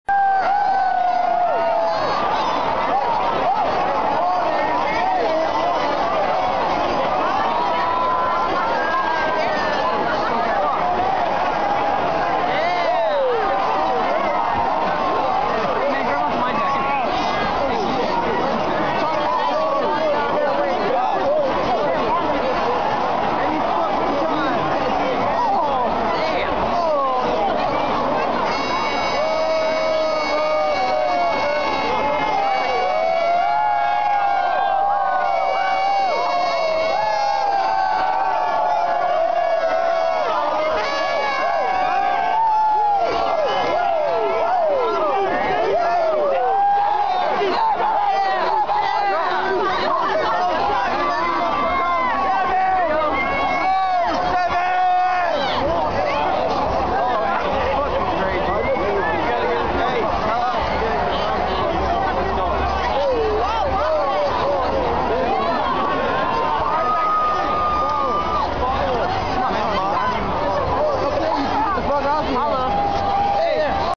(REUPLOADED) las vegas new years
Reuploaded from ilikecrxs because he deleted his channel.
Original Upload Date: January 7, 2007
Original Description: trying to walk through people